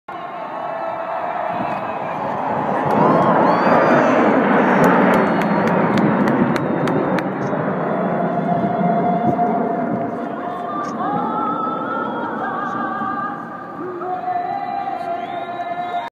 Thunderbirds Flyover at the Las Vegas Motor Speedway - NASCAR Kobalt Tools 400 - 2013
the last flyover we'll see in a long time, but still very cool! The Thunderbirds did an awesome job!